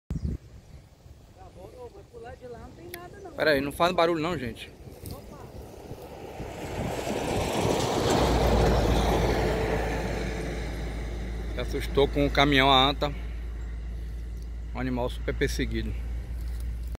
Anta andando de dia sem rumo.
Por conta das queimadas e desmatamentos os animais estão vagando de dia na margem das rodovias. Triste ver os animais desamparados dessa forma aqui no Pará.